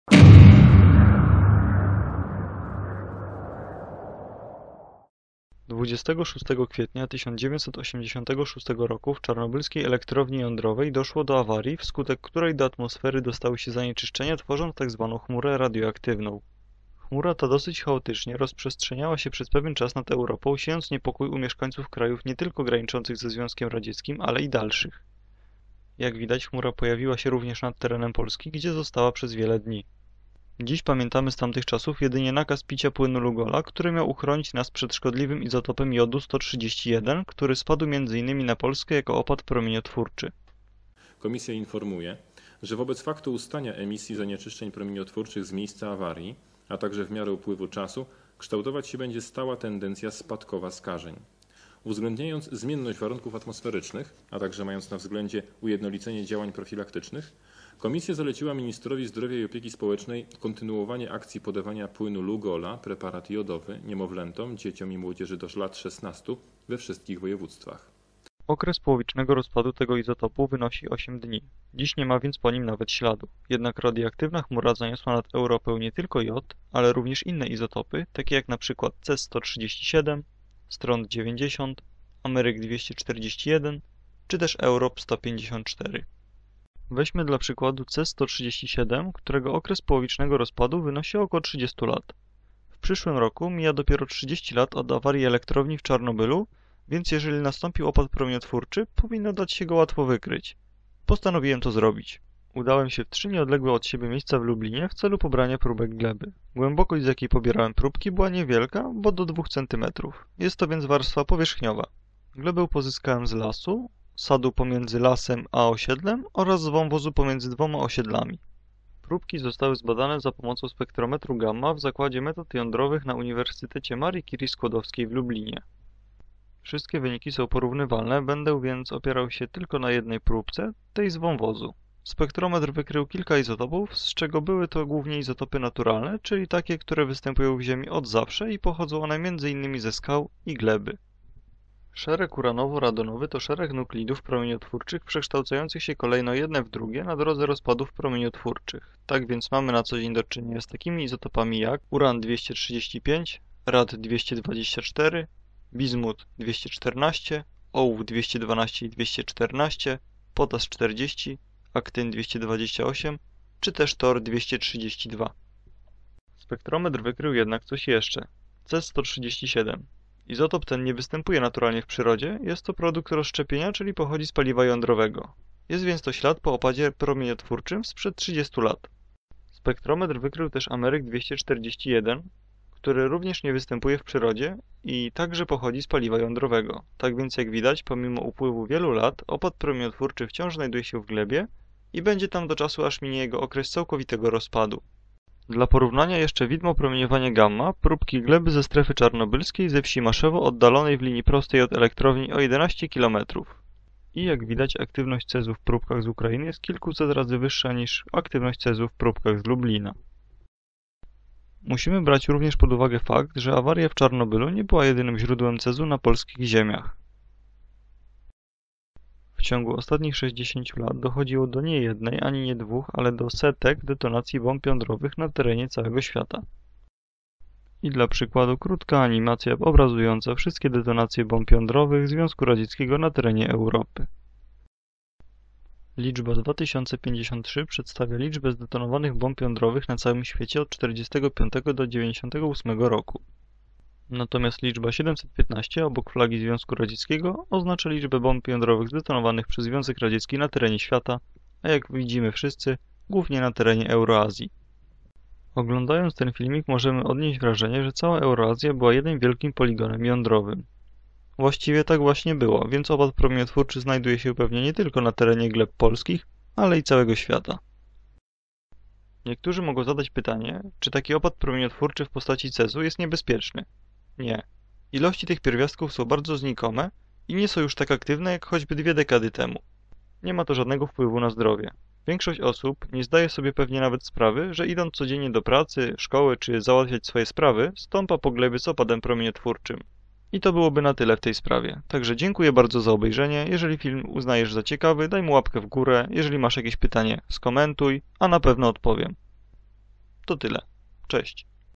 Opad radioaktywny w Polsce - analiza gleby
Czy możemy znaleźć w dzisiejszych czasach w glebie ślady opadu promieniotwórczego z awarii w Czarnobylu lub detonacji bomb jądrowych? Sprawdźmy to!